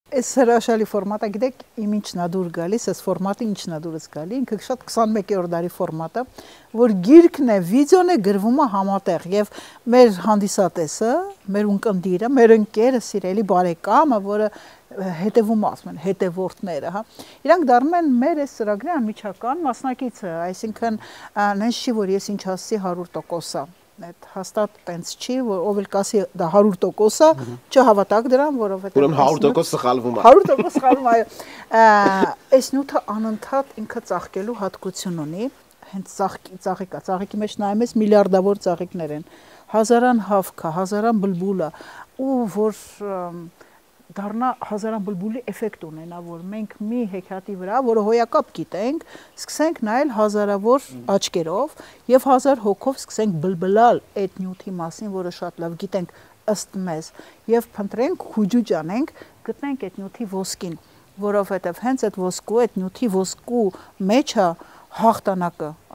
Դրախտը / Նախերգանք / Paradise part 1
This is a video about Fairy Tales.
Նախագիծը կոչված է վեր հանելու հայկական ավանդազրույցներում, հեքիաթներում և ծիսակարգերում թաքնագրված խուրհուրդների, խորհրդանիշերի, իմաստությունների ու մեր օրերում դրանց արդիականության, կարևորության ու կիրառելիության զուգահեռները: